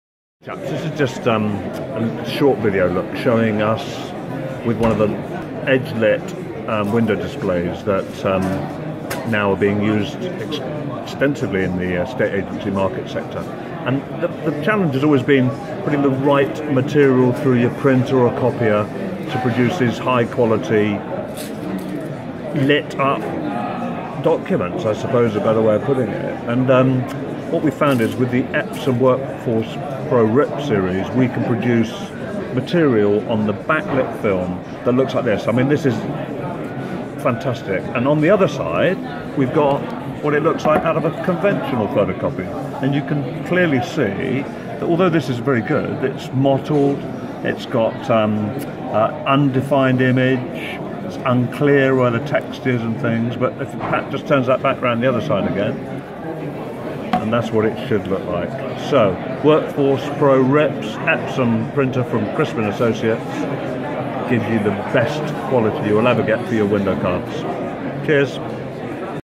Affordable and high quality estate agents window cards
Using the superb Epson Workforce Pro RIPS technology from Crispin, to print cost-effective window cards for edge-lit window displays